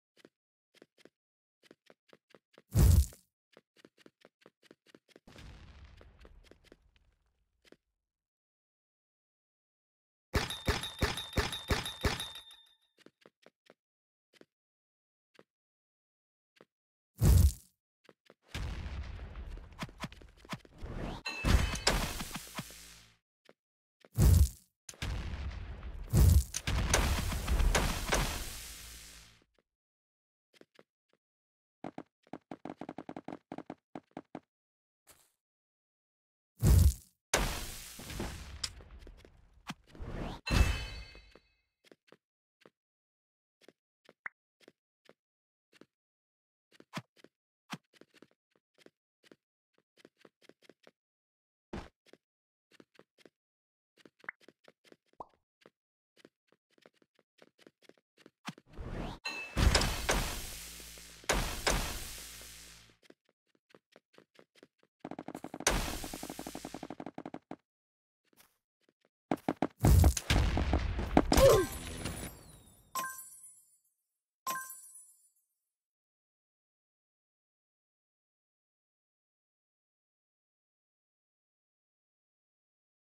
1v1 in a public match
i dont know why i decided not to take the black holes or mine the other lucky air drop